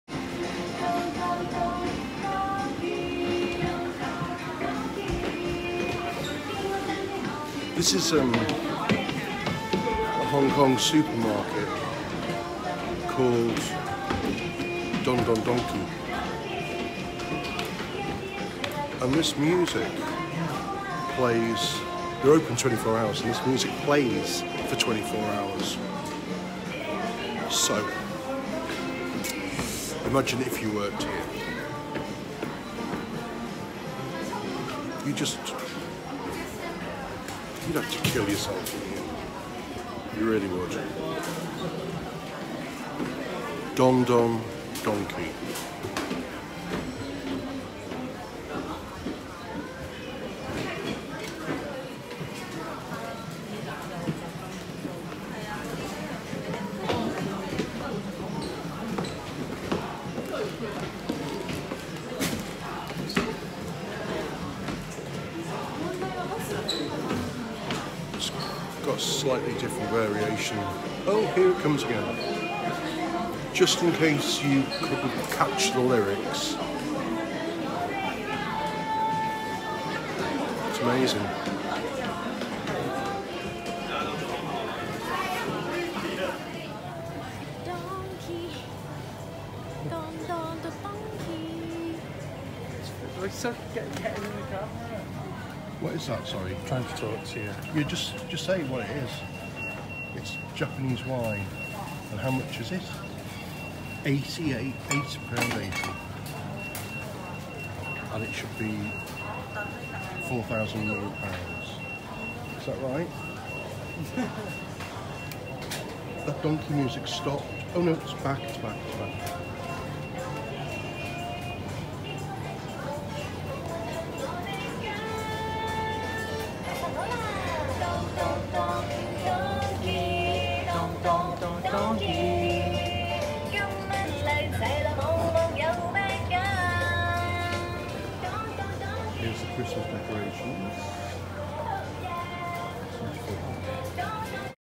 DON DON DONKI SUPERMARKET IN HONG KONG !
Kitschy supplier of almost everything, comes with a repetitive background tune !